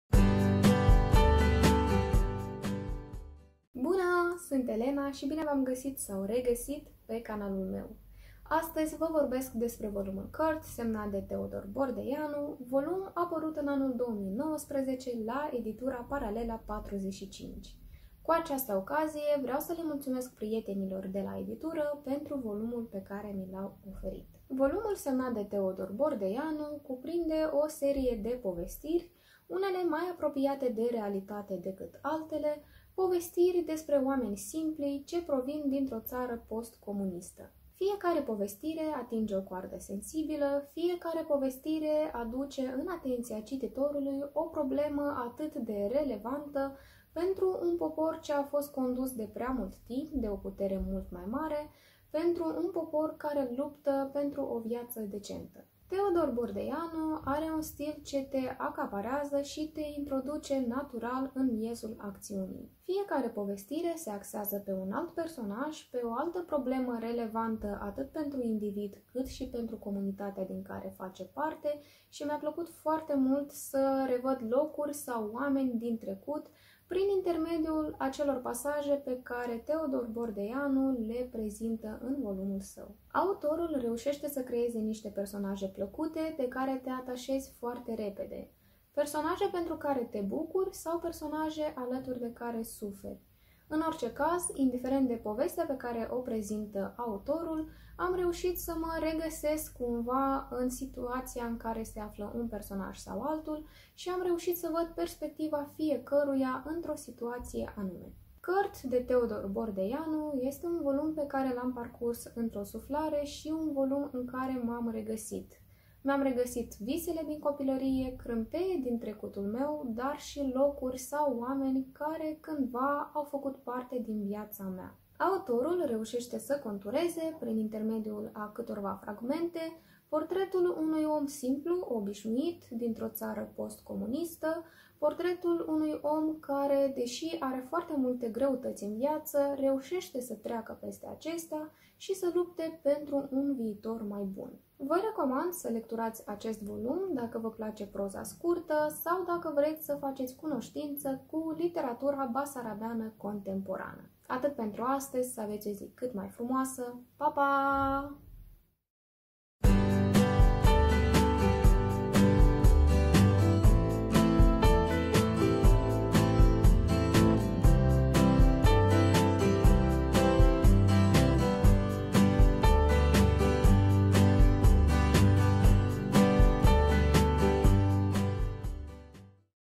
Kurt de Teodor Bordeianu - Recenzie (Spoiler Free)
Fragmente din viața unor oameni care muncesc din greu pentru un trai mai bun. Povești despre locuri demult uitate. Amintiri despre vremuri demult apuse. Kurt de Teodor Bordeianu este un volum plin de amintiri, care trezește nostalgia și dorința de a mai revedea o dată copilăria, dar, de data aceasta, prin ochii adultului care ești astăzi.